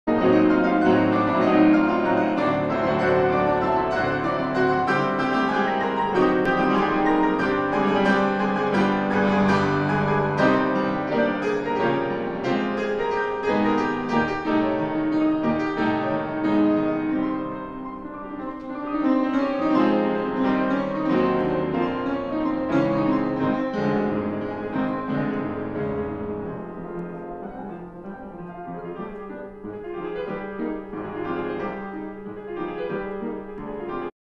Belling Duo 1.MOV
Belling Duo at Piano Time with Bluthner model 10 and Wertheim AW185